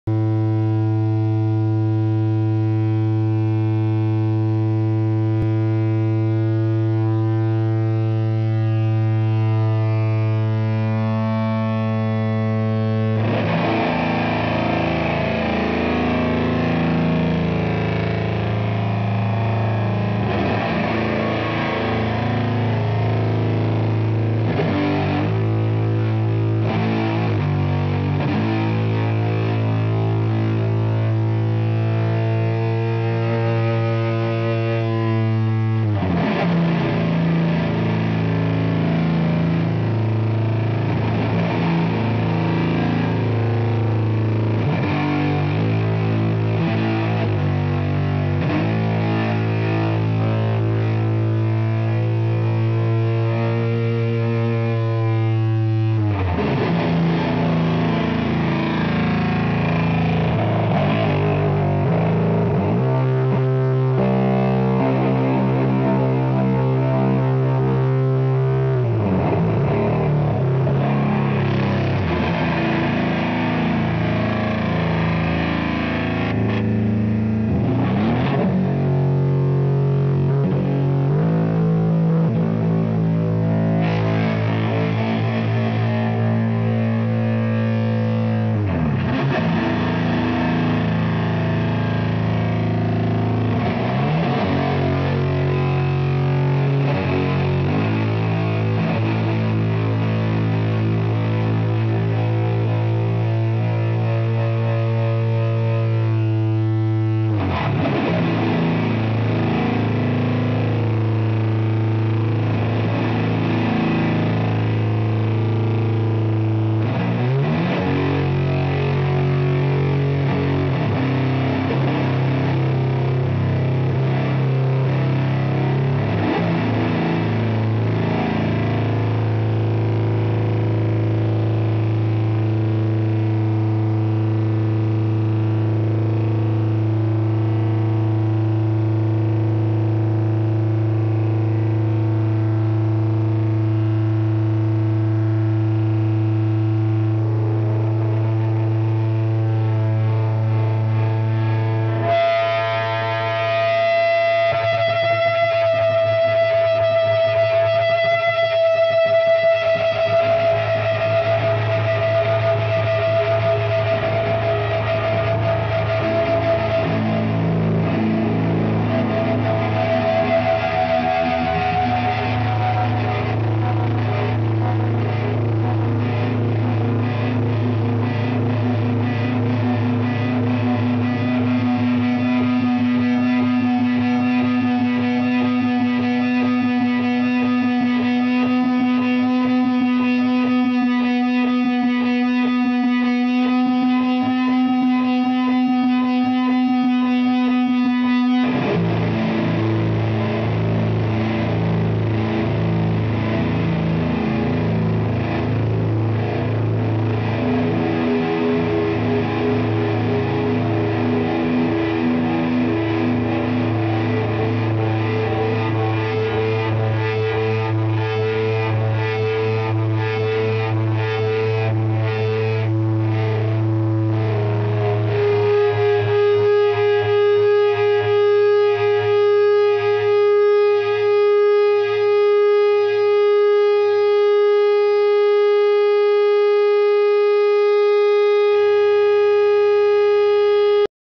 Orange OR120 clone (Tankamp Plutonaut) & Green Matamp emulator
Gear. Amps:
- Tankamp "Plutonaut" based on '72 OR120 schematic \w mods - 2x12 cab
- Marshall JTM DIY - 4x12 cab

Pedals:
- Distortion+
- Green Matamp emulator
- EHX LPB-1
- Noname stereo chorus used also as Y-splitter

Guitar:
- Cort EVL Z2 \w EMG's 81-85.